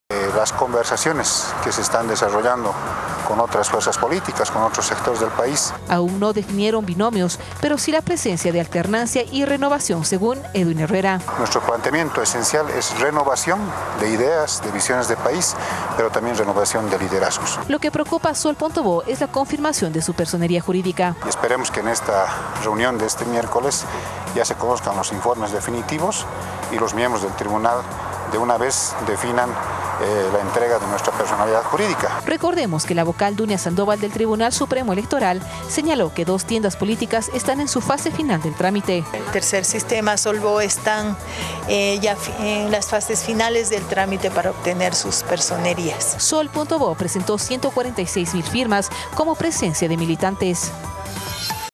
RED UNO Sol bo 17 9 2018
Sol Bol espera que en esta semana el Tribunal Supremo Electoral le haga entrega de la personalidad jurídica, considerando que la vocal Dunia Sandoval adelantó que Tercer Sistema y Sol Bol están en la fase final de su trámite para lograr su objetivo.